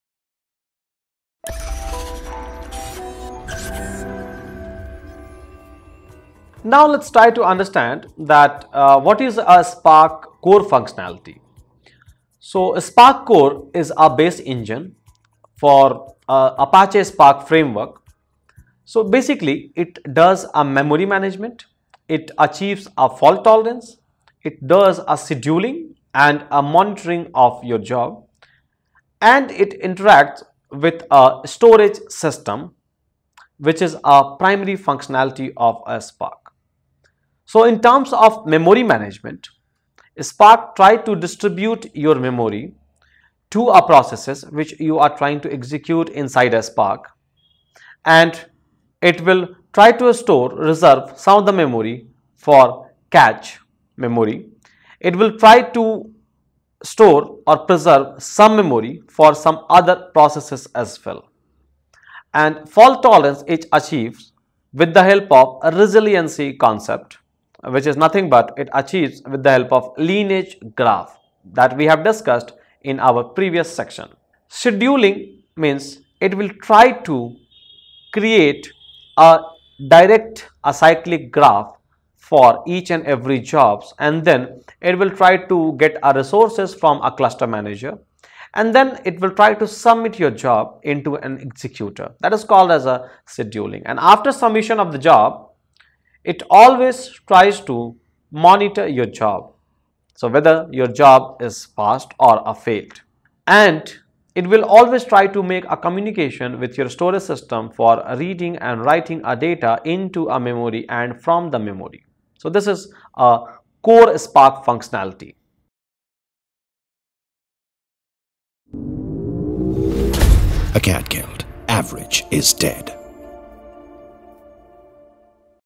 What is Spark Core Function | Hadoop Interview Questions and Answers
Welcome back to Apache Spark interview questions and answers powered by Acadgild. Here in this video, Mr. Sudhanshu, a Data Scientist, will explain you the Hadoop interview questions and answers specifically on apache spark. If you have missed the master video of interview question and answers kindly, click the following link.
In this video the mentor will explain the components of spark ecosystem.
What is Spark Core Function?
Spark Core is a base engine of Apache Spark framework, basically it does memory management, fault tolerance, scheduling and monitoring jobs, interacting with store systems are primary functionalities of spark